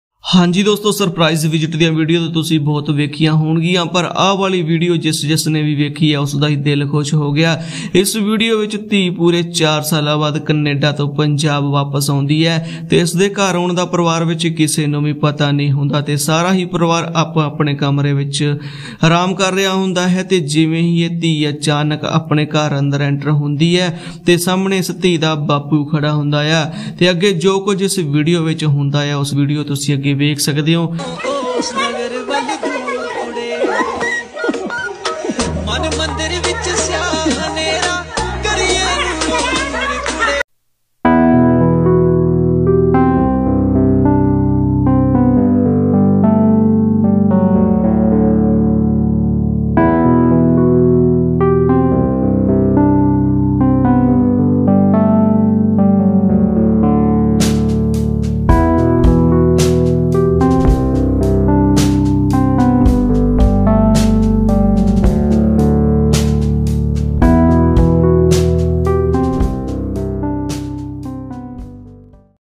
ਕੈਨੇਡਾ ਤੋ ਘਰ ਆਕੇ ਇਸ ਧੀ ਨੇ ਆਪਣੇ ਪਰਿਵਾਰ ਨੂੰ ਦਿੱਤਾ ਇੱਕ ਅਨੌਖਾ ਹੀ ਸਰਪ੍ਰਾਇਜ਼ | surprise visit to punjab
ਕੈਨੇਡਾ ਤੋ ਘਰ ਆਕੇ ਇਸ ਧੀ ਨੇ ਆਪਣੇ ਪਰਿਵਾਰ ਨੂੰ ਦਿੱਤਾ ਇੱਕ ਅਨੌਖਾ ਹੀ ਸਰਪ੍ਰਾਇਜ਼ | surprise visit to punjab | surprise visit to India | Punjab to Canada | Canada visa